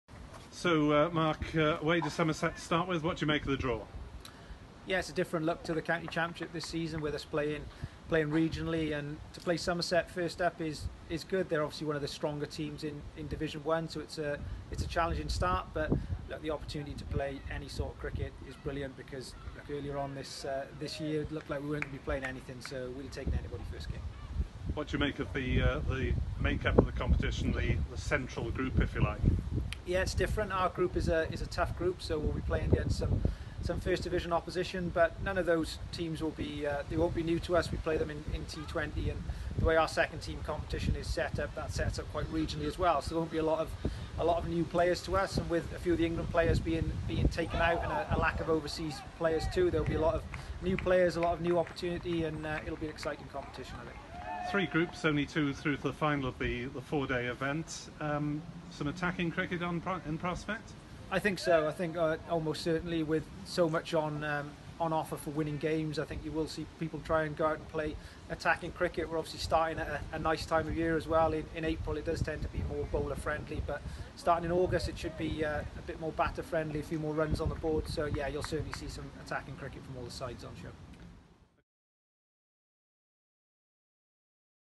Mark Wallace fixtures reaction
Glamorgan's Director of Cricket, Mark Wallace, reacts to the announcement of the Bob Willis Trophy fixtures.